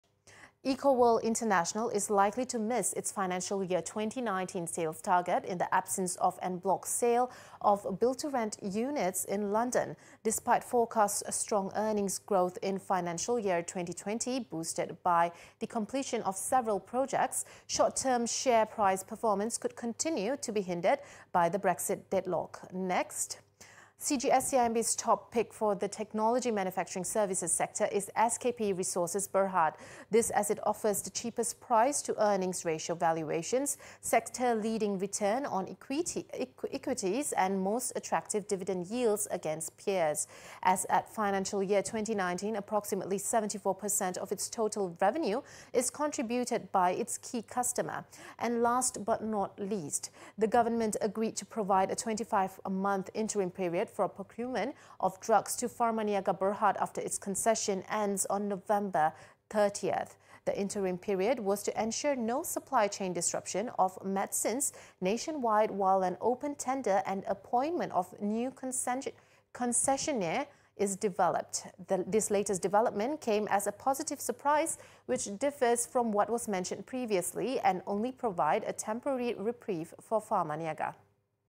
Bizline | Stocks To Watch
Stocks to watch for today include Eco World International, SKP Resources Berhad and Pharmaniaga Berhad.